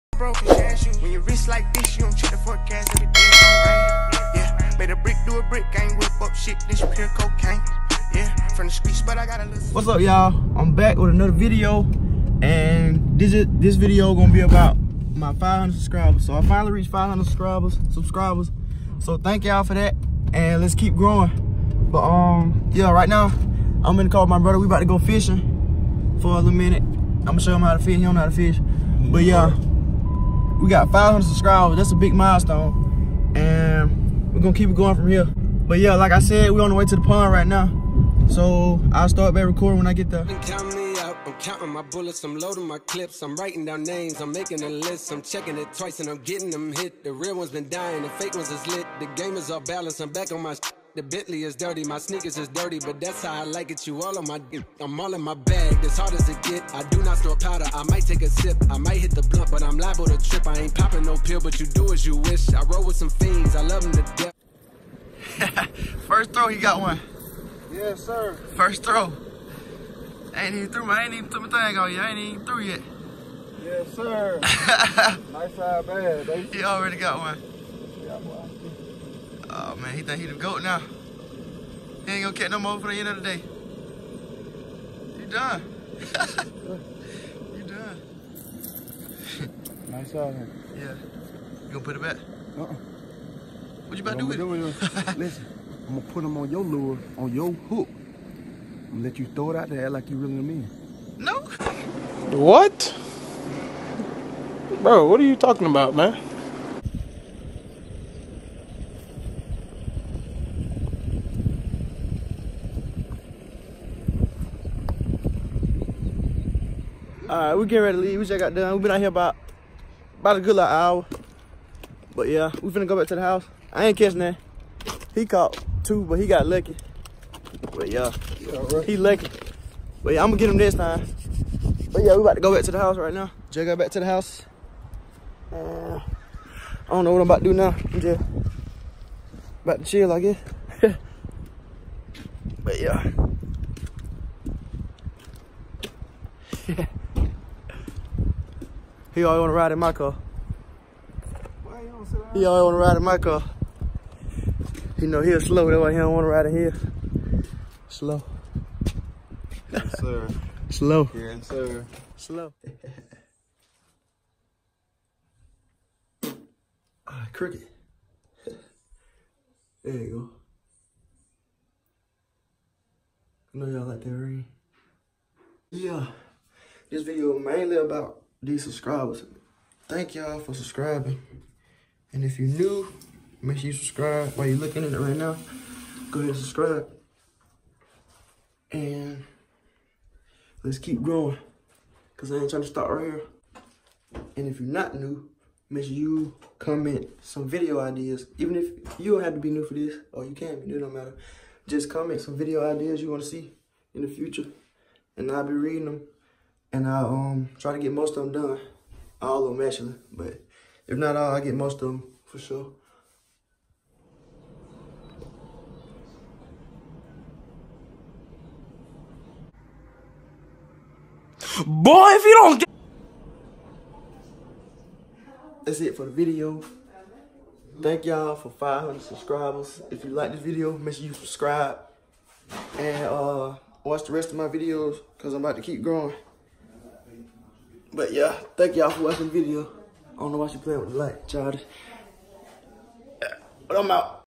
500 SUBSCRIBER SPECIAL📈| + FISHING WITH BROTHER 🎣| HE CAUGHT A FISH ON THE FIRST CAST 🐠
500 SUBSCRIBER SPECIAL📈| + FISHING WITH BROTHER 🎣| HE CAUGHT A FISH ON THE FIRST CAST 🐠